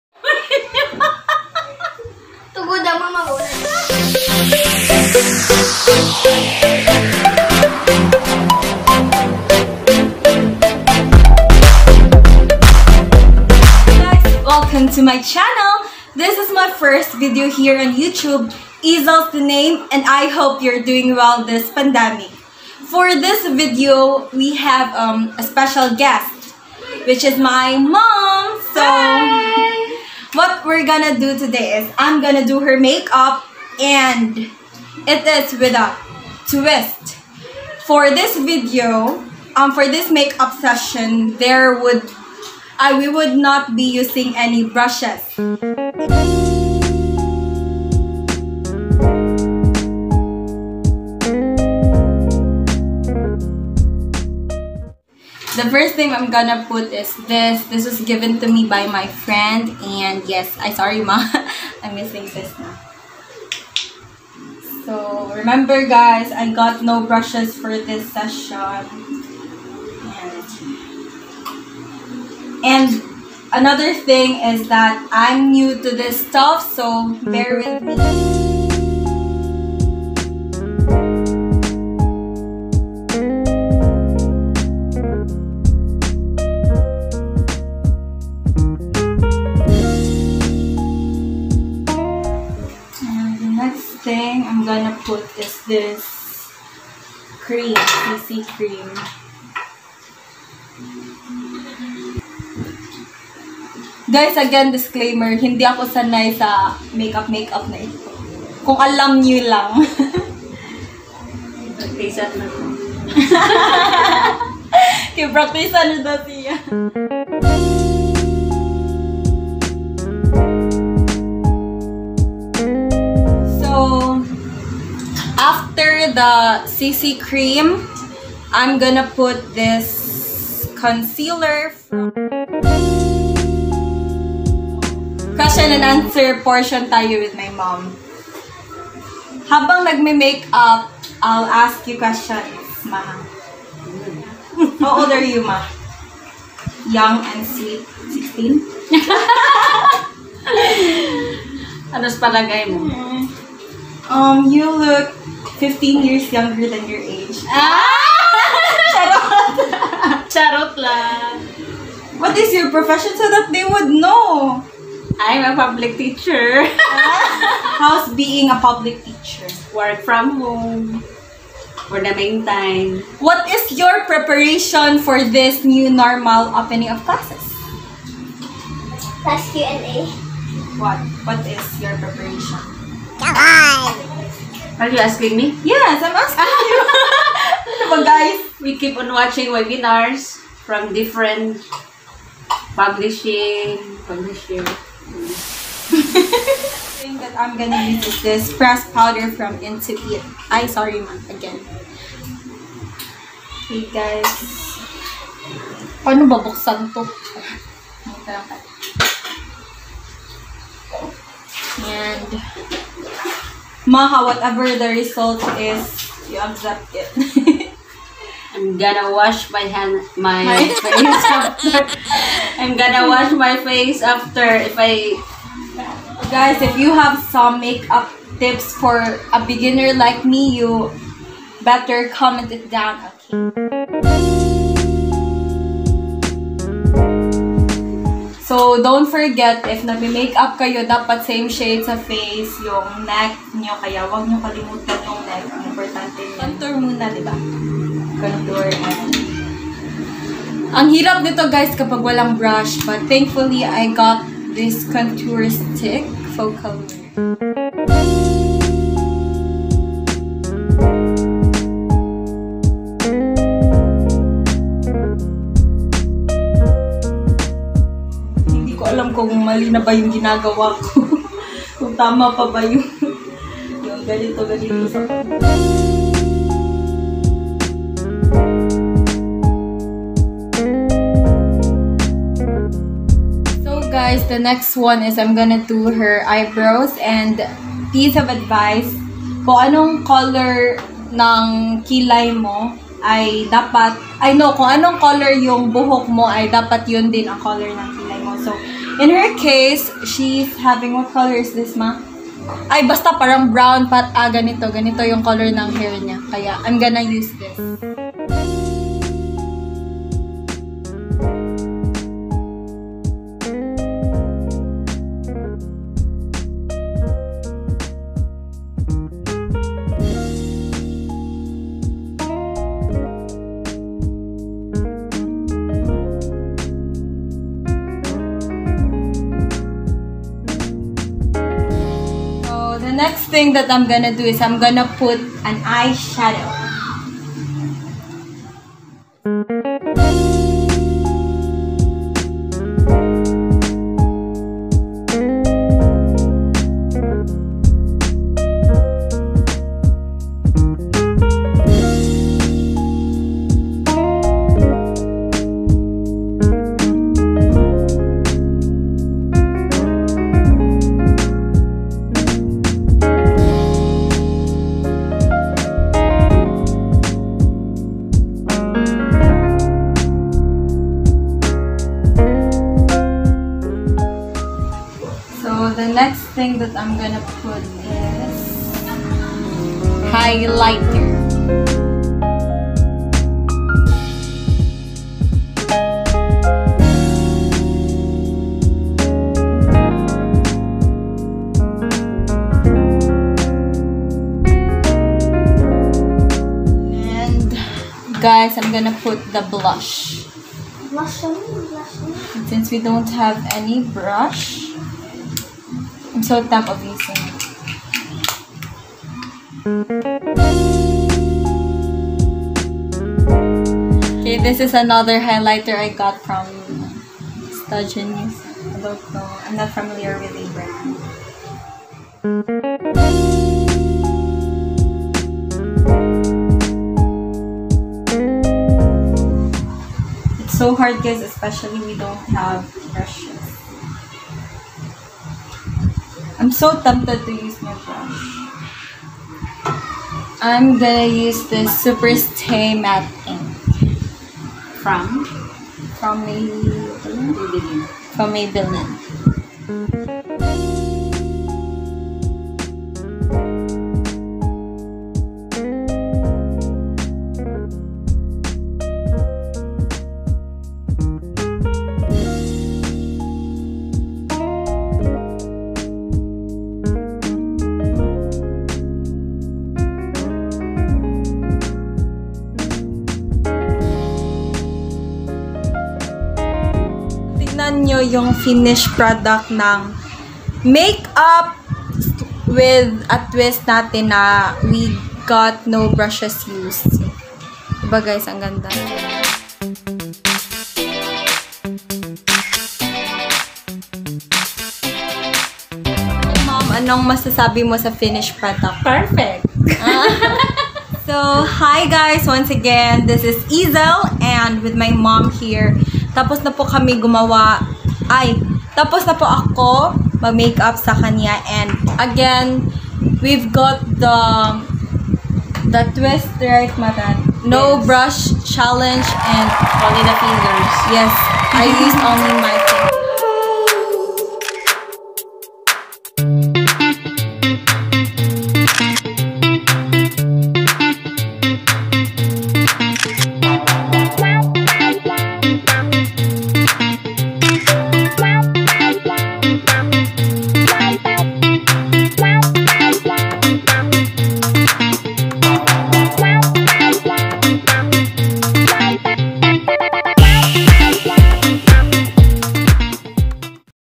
FIRST VLOG! Doing my Mom's Make-Up (With a twist)
Hi guys! Sit back and relax, HAVE FUN.
No make-up brush challenge, is a make-up session with no brushes used during the entire vlog. Ft. here with me is my Mom.
Keep safe :)